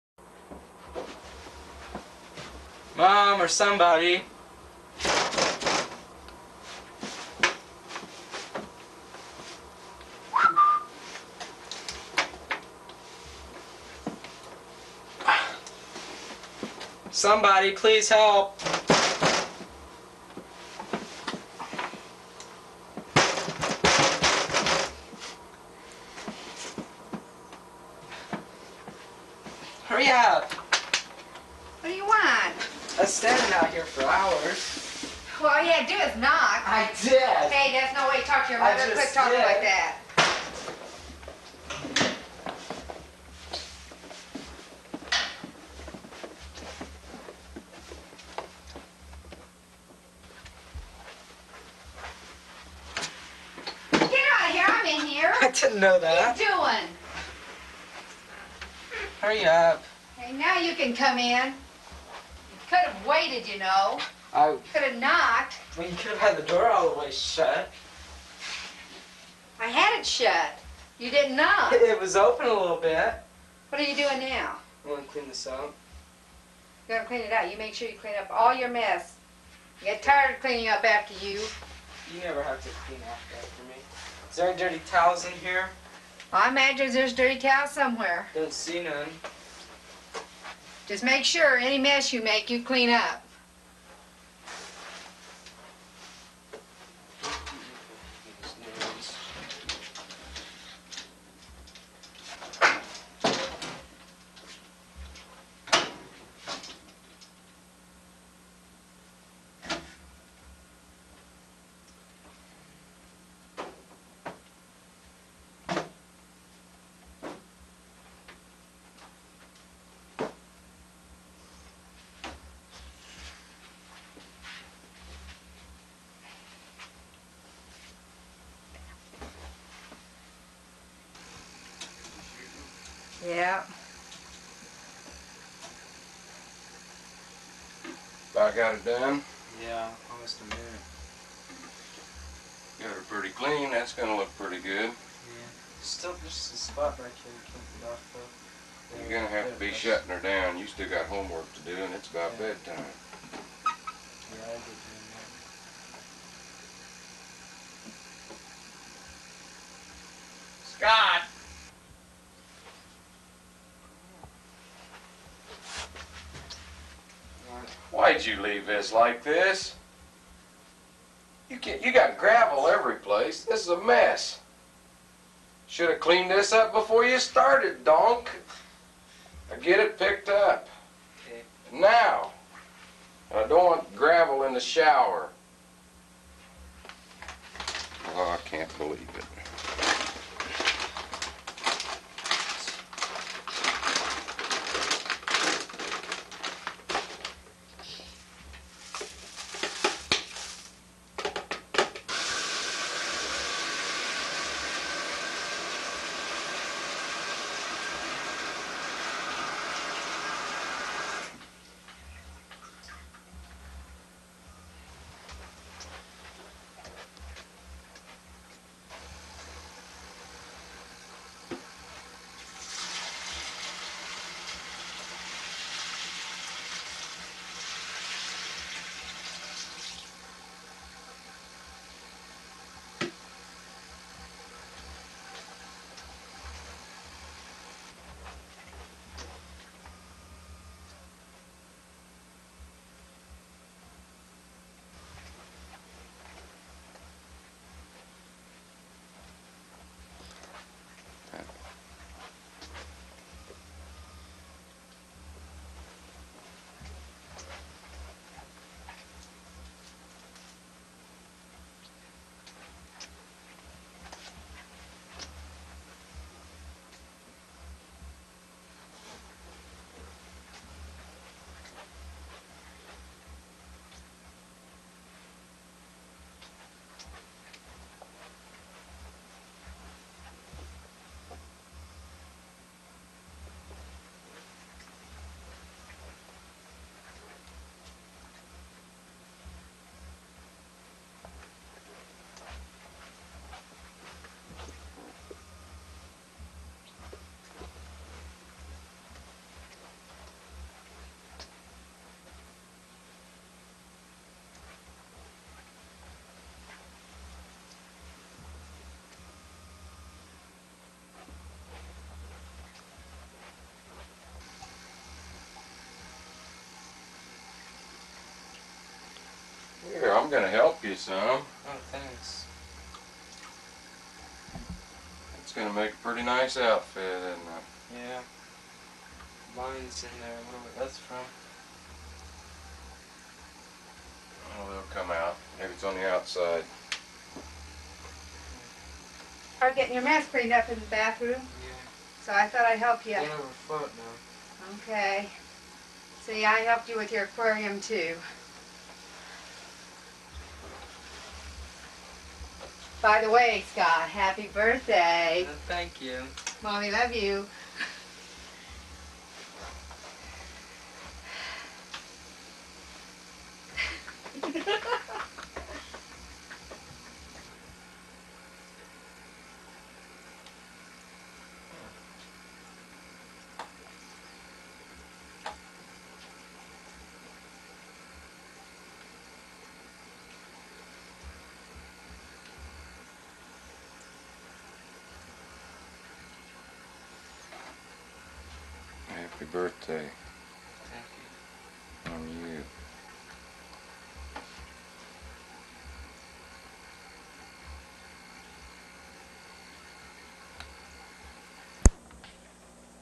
Fish Story - My 16th Birthday
1984 - My mom, dad and I take movie making to a whole new place.  What the captivating tail (referring to my big butt) of a new aquarium getting set up.